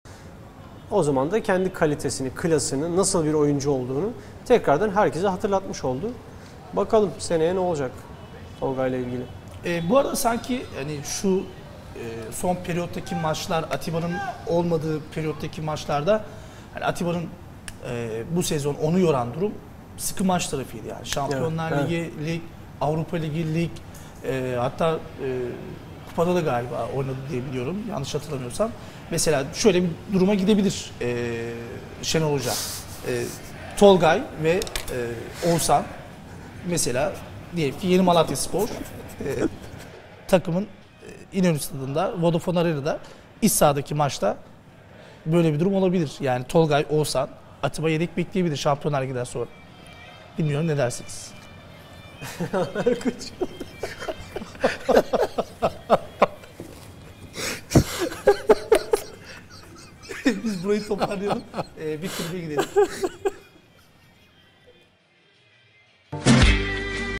Önder Özen ve Uğur Boral Canlı Yayında Gülme Krizine Girdi
Sunucunun dirseği kayınca gülme krizine girdiler :D